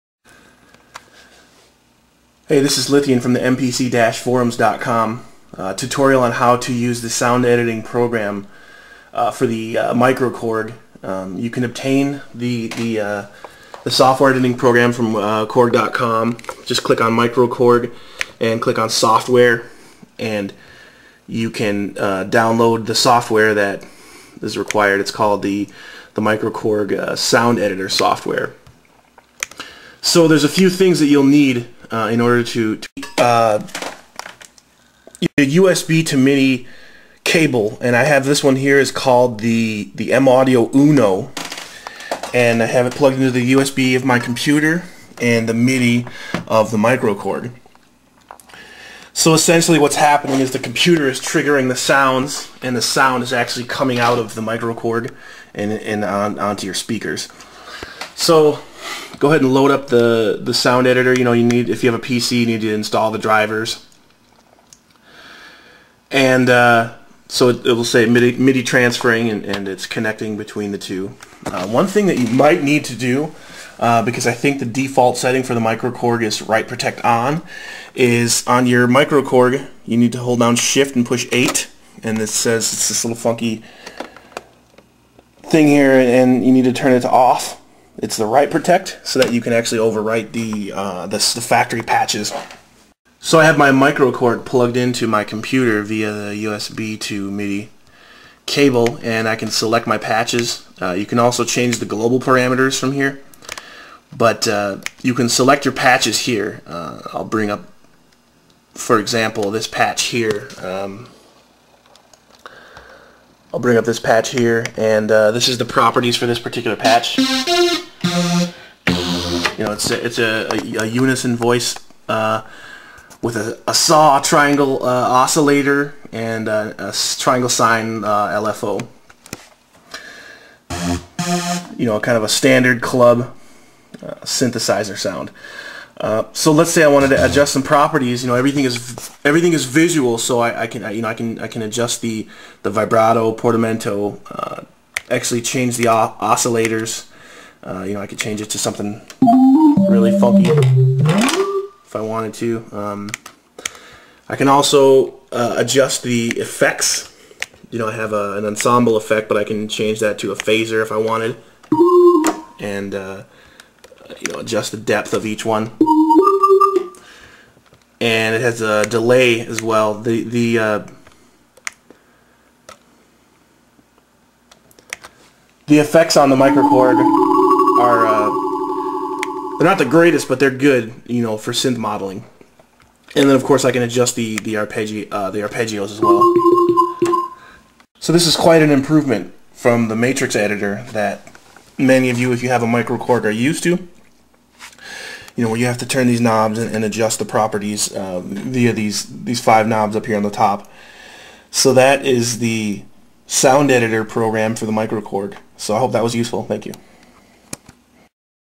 MicroKorg synth modeling with the software editing program
A quick demonstration of how you can use the Korg software to edit patches on your microkorg